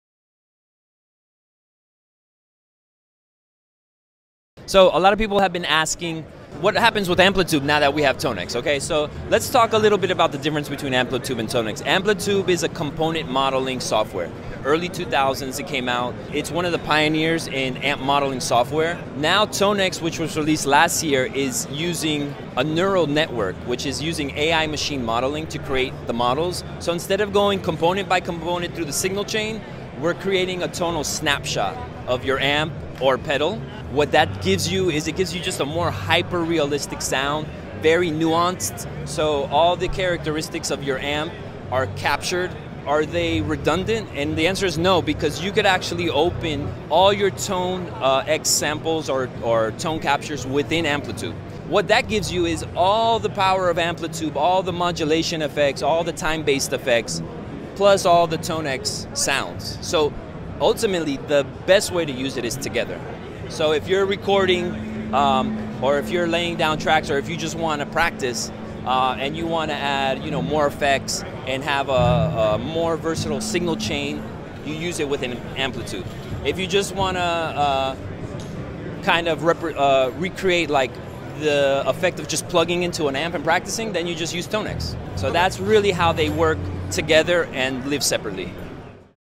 Talk w/ @ikmultimedia about difference between Amplitube and Tonex
Stopped by @ikmultimedia booth to talk about their Tonex plugin and pedals.